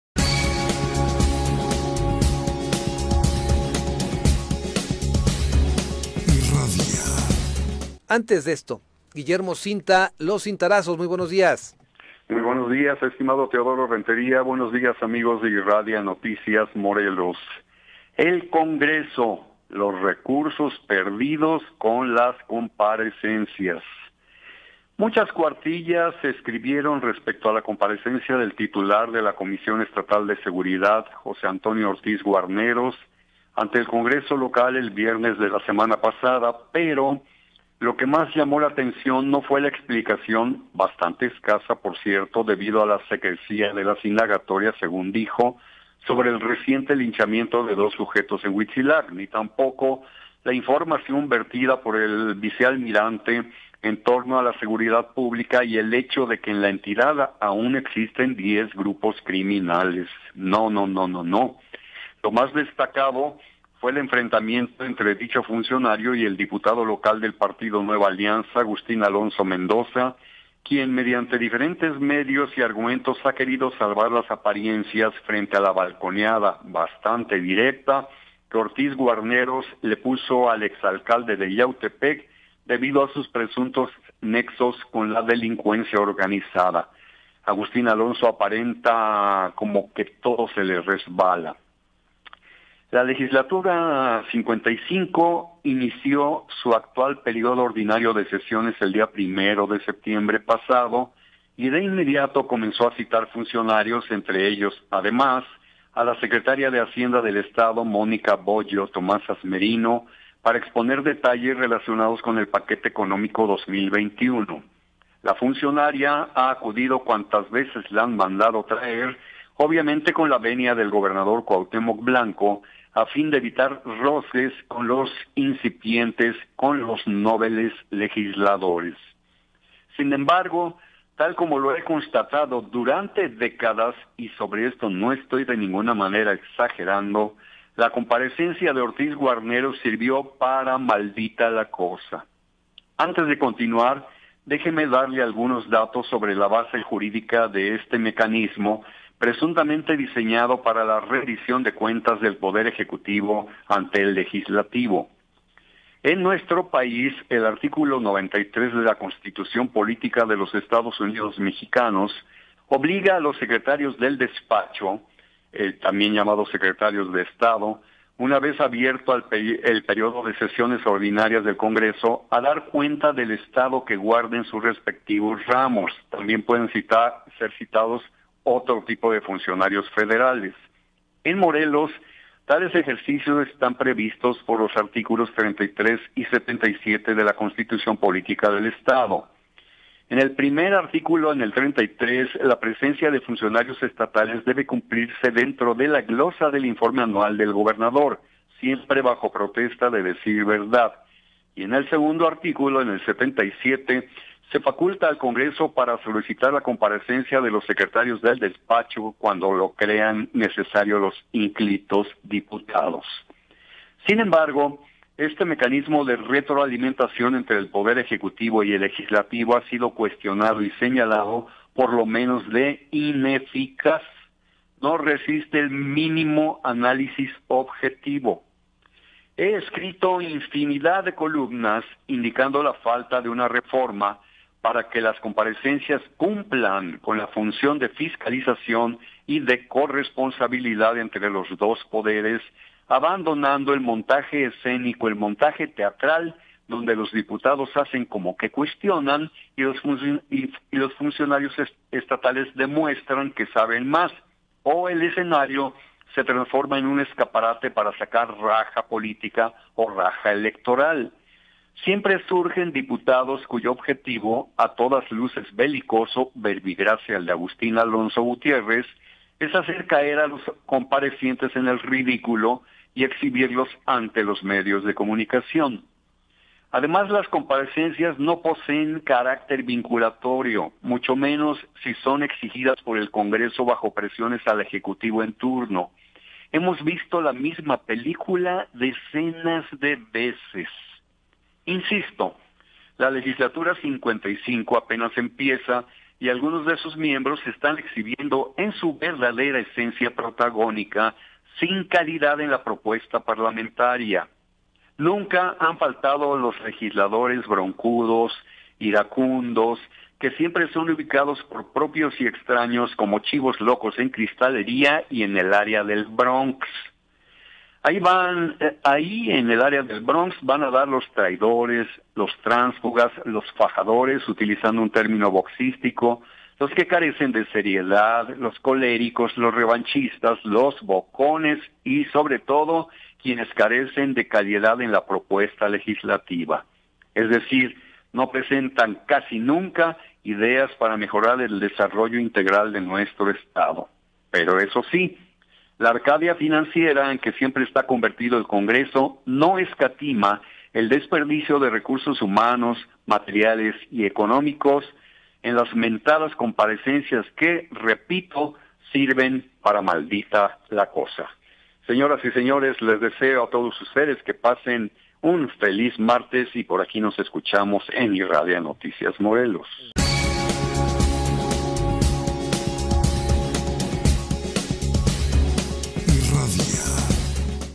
Guillermo Cinta | LOS CINTARAZOS [12-OCT-2021] IRRADIA NOTICIAS MORELOS
El congreso, los recursos perdidos con las comparecencias.